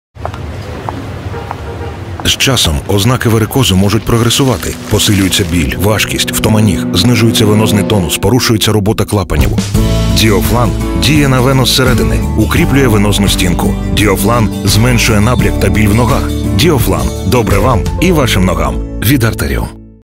PEOPLE & Диофлан
Актриса кастинг-агентства PEOPLE - Татьяна снялась в видео рекламе "Диофлан"!